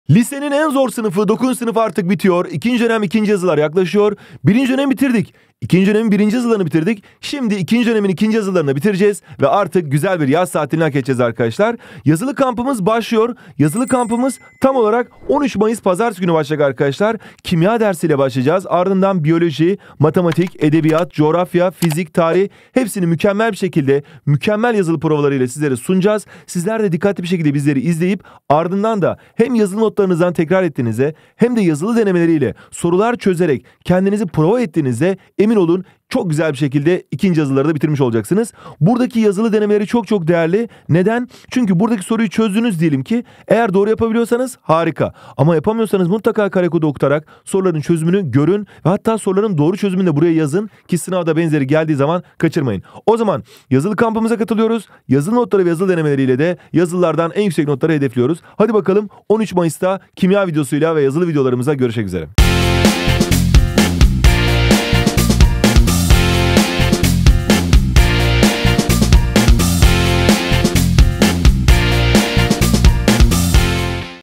2. DÖNEM 2. YAZILILAR YAKLAŞIYOOOR! 📢
13 Mayıs’ta 2.Dönem 2.Yazılı Kampına başlıyoruz! 🎉 tonguç, yazılı döneminde de yanında! Yazılılarında çıkabilecek konularla ilgili, bilmen gereken önemli noktalar ve sana özel hazırlanmış yazılı prova soruları ile birlikte seni yazılılara mükemmel bir şekilde hazırlıyoruz!

9. Sınıf 2. Dönem Süper 2'li Yazılıya Hazırlık Seti (Hediyeli) 👉🏻